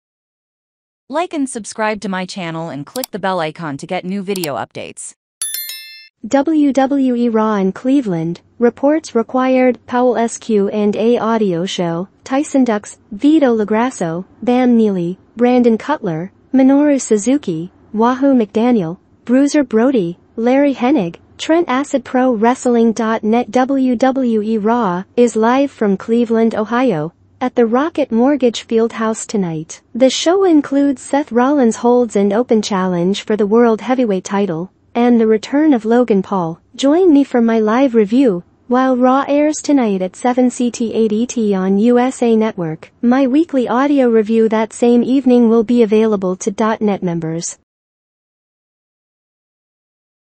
WWE RAW IN CLEVELAND (REPORTS REQUIRED). POWELLS Q&A AUDIO SHOW. TYSON DUX. VITO...
WWE Raw in Cleveland (Reports Required). Powells Q&A Audio Show. Tyson Dux. Vito LoGrasso. Bam Neely. Brandon Cutler. Minoru Suzuki. Wahoo McDaniel. Bruiser Brody. Larry Hennig. Trent Acid – Pro Wrestling Dot Net

Join my live review as Raw airs tonight at 7CT/8ET on USA Network.